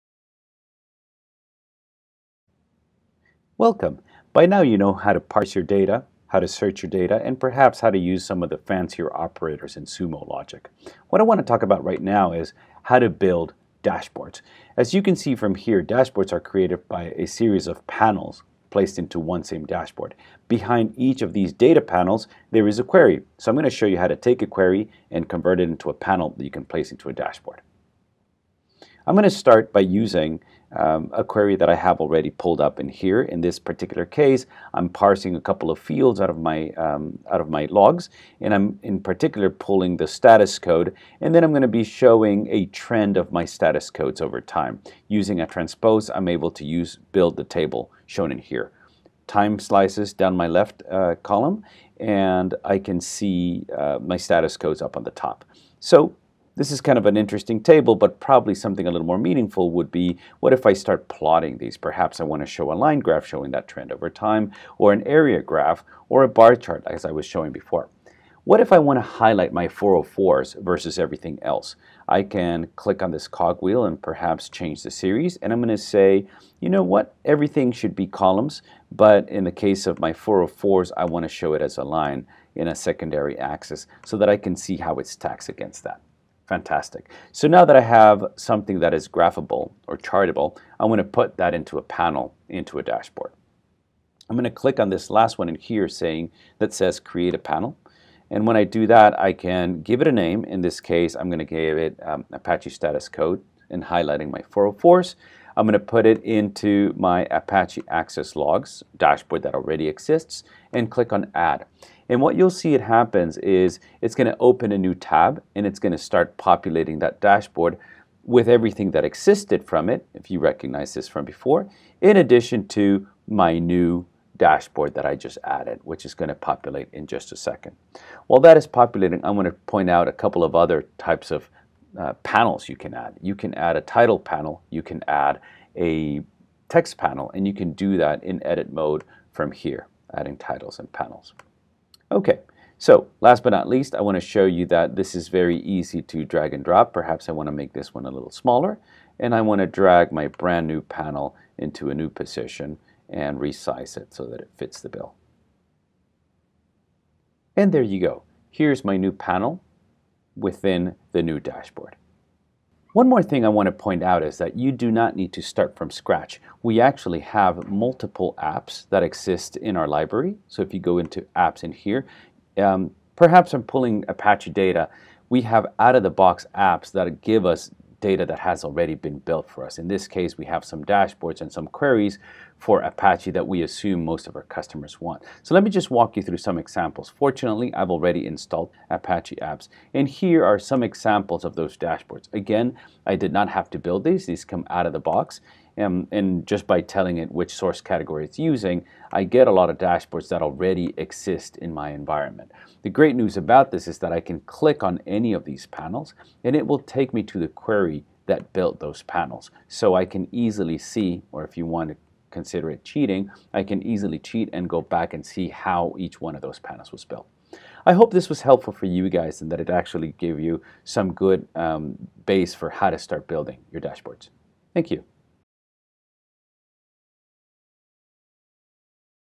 Getting Started with Sumo Logic: Creating Dashboards
Learn how to create Sumo Logic Dashboards to bring your info to light.